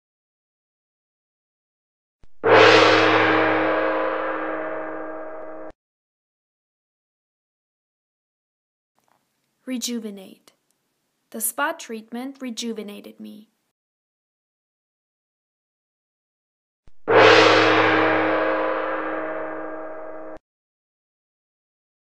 How To Pronounce 'REJUVENATE' | Ask Linda! | Pronunciation
In this episode, Linda takes on the pronunciation of the word "rejuvenate". Enjoy the episode!